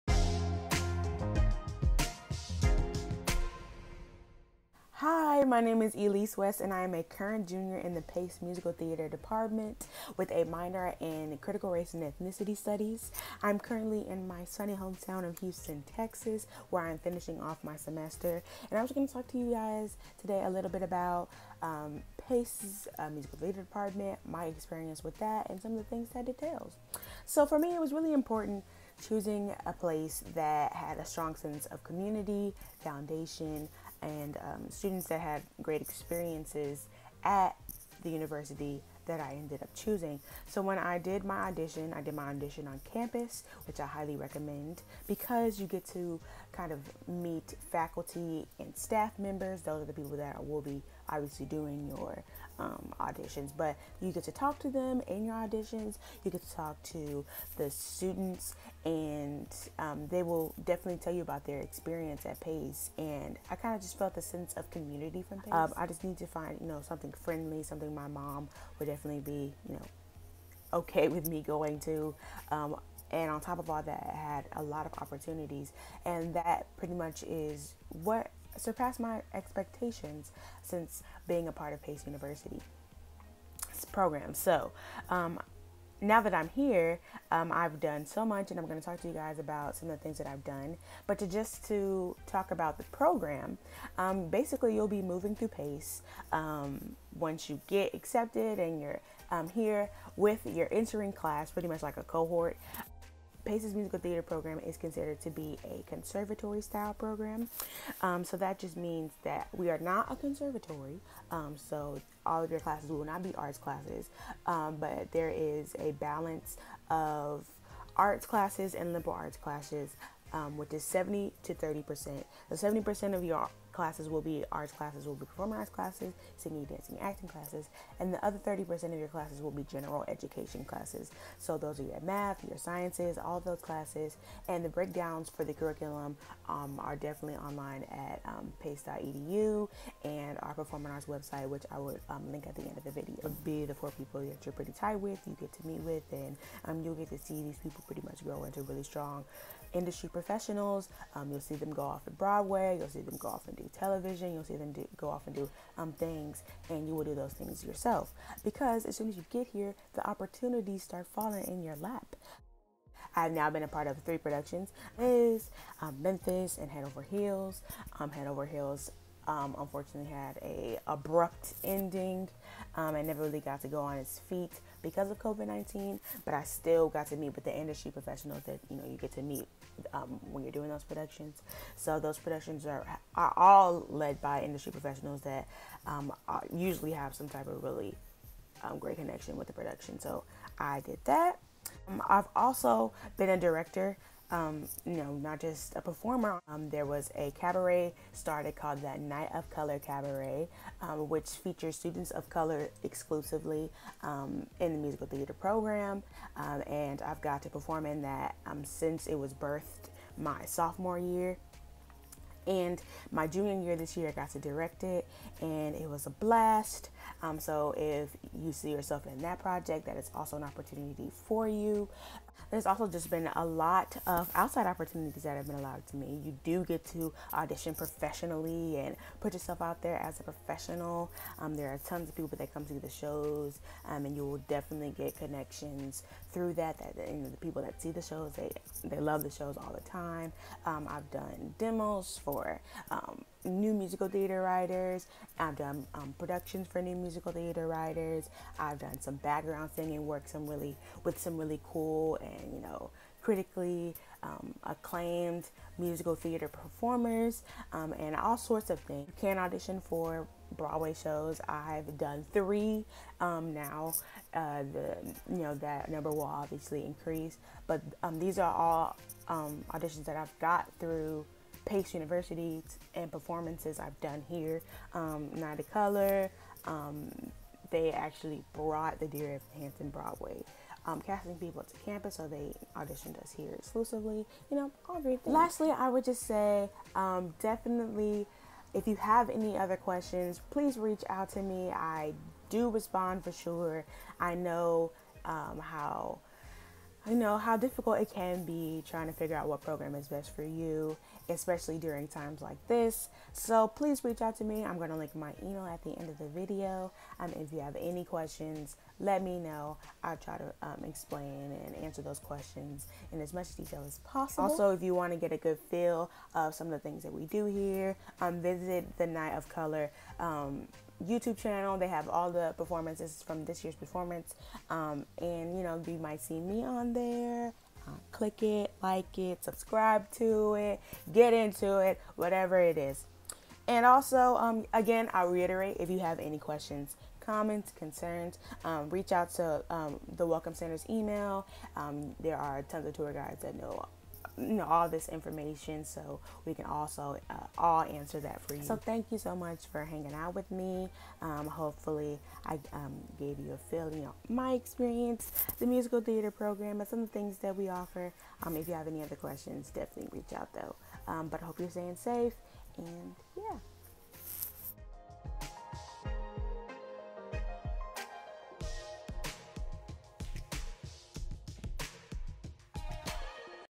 Pace Performing Arts: BFA Musical Theatre
Pace's Performing Arts Program, or as we call it PPA, is part of the Dyson College of Arts and Sciences. Tour Guide Elyce shares information about the BFA Musical Theatre program along with her own personal experiences!